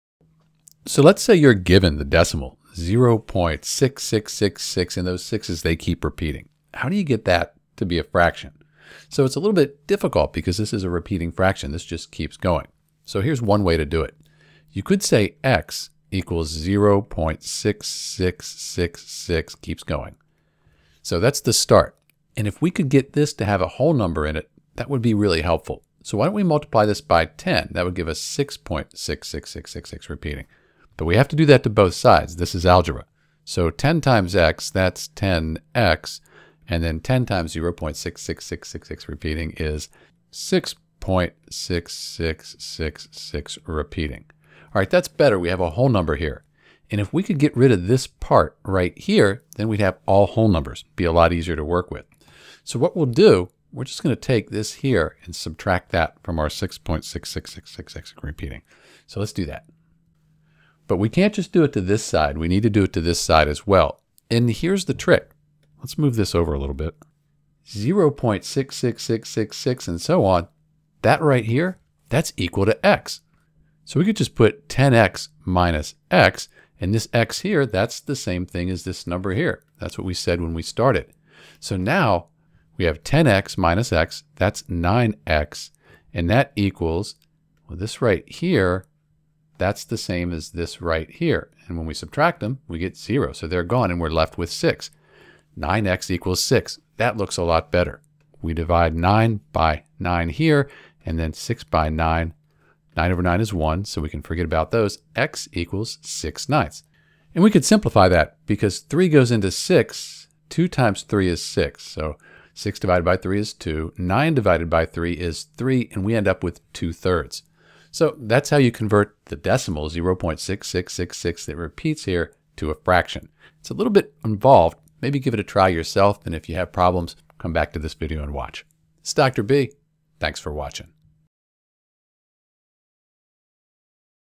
How to write the decimal 0.6666... (repeating) as a fraction!
To find the fraction that’s equivalent to the decimal 0.6666..., we first set x equal to 0.6666. This helps us keep track of the repeating decimal. Next, we multiply both sides of the equation by 10, which gives us 10x = 6.6666. We do this to shift the decimal point so we can subtract out the repeating part.

Now, we subtract x from 10x. On the right side, we’re subtracting 0.6666... from 6.6666..., which leaves us with just 6. On the left side, subtracting x from 10x leaves us with 9x.

At this point, we have 9x = 6, and to find x, we divide both sides by 9. That gives us x = 6/9, which simplifies to 2/3. So, 0.6666... is equal to 2/3. Each step is designed to eliminate the repeating decimal and convert it into a fraction.